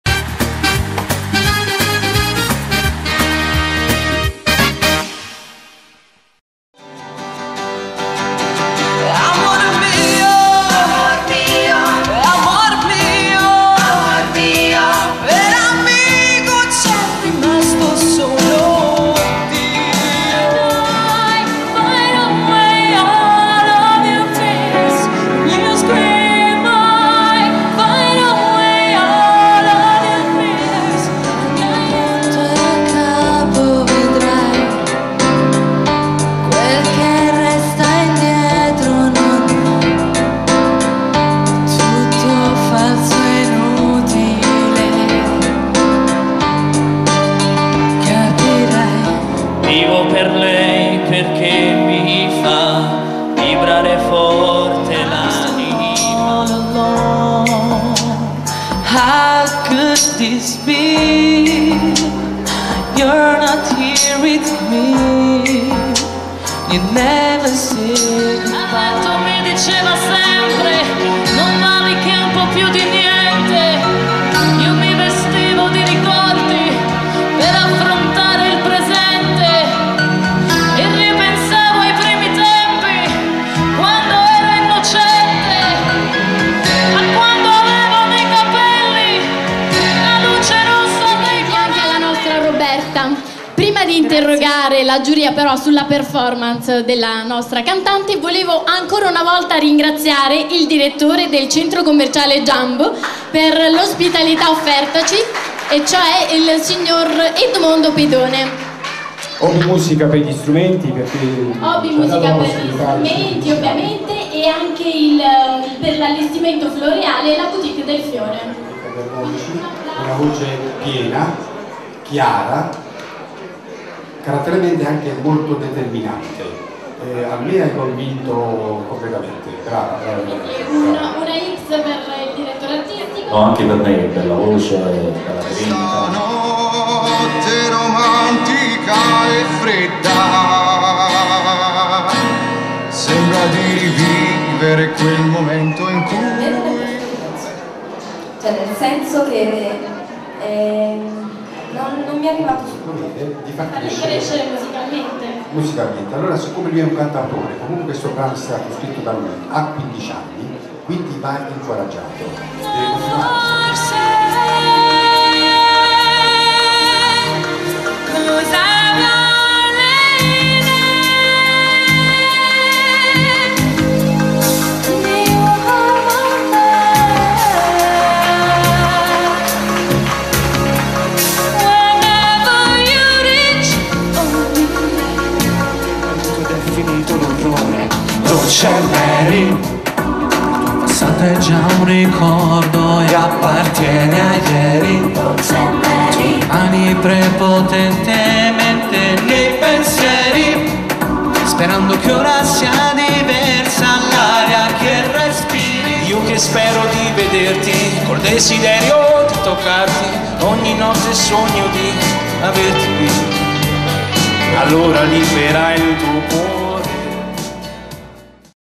Una Voce in Campania TV - 2010 - Audizioni al Jambo
La nuova web tv del Festival Una Voce in Campania
A cura dell'Associazione Musicale "violinista Lina Russo Quattromani"

Audizioni al Jambo - X° edizione del festival, audizioni al Centro Commerciale Jambo di Trentola Ducenta

Direzione Artistica: Rino Quattromani
Ideazione e Realizzazione: Rino Quattromani, Luca Bianco
Regia: Luca Bianco, Giovanni Quattromani
- Festival Canoro "Una Voce in Campania" -
Festival per cantanti, gruppi, cantautori